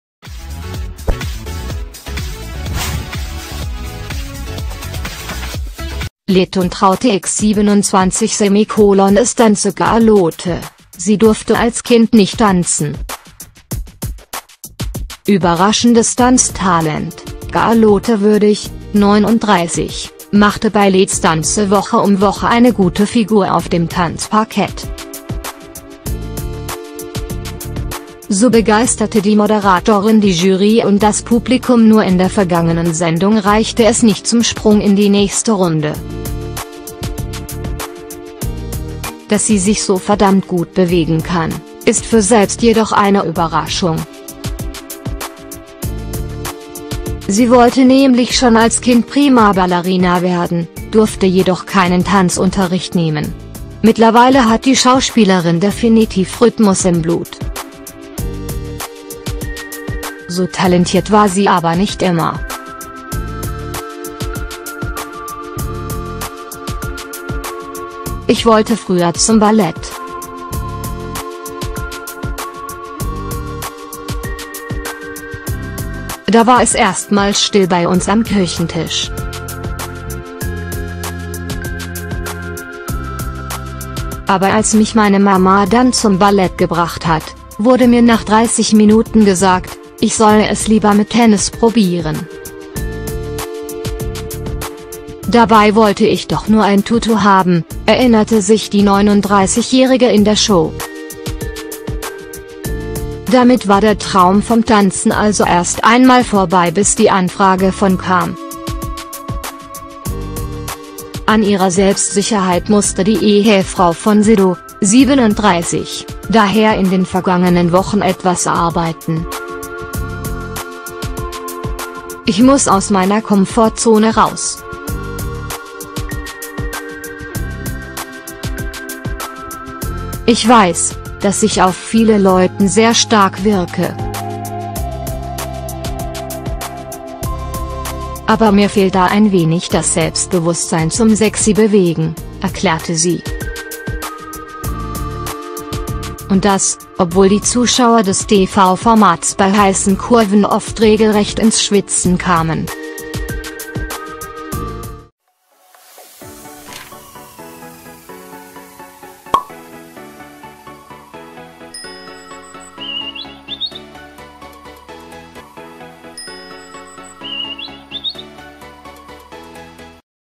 "Let's Dance"-Charlotte: Sie durfte als Kind nicht tanzen!
Überraschendes Tanztalent: Charlotte Würdig (39) machte bei Let's Dance Woche um Woche eine gute Figur auf dem Tanzparkett. So begeisterte die Moderatorin die Jury und das Publikum – nur in der vergangenen Sendung reichte es nicht zum Sprung in die nächste Runde. Dass sie sich so verdammt gut bewegen kann, ist für Charlotte selbst jedoch eine Überraschung. Sie wollte nämlich schon als Kind Prima Ballerina werden, durfte jedoch keinen Tanzunterricht nehmen!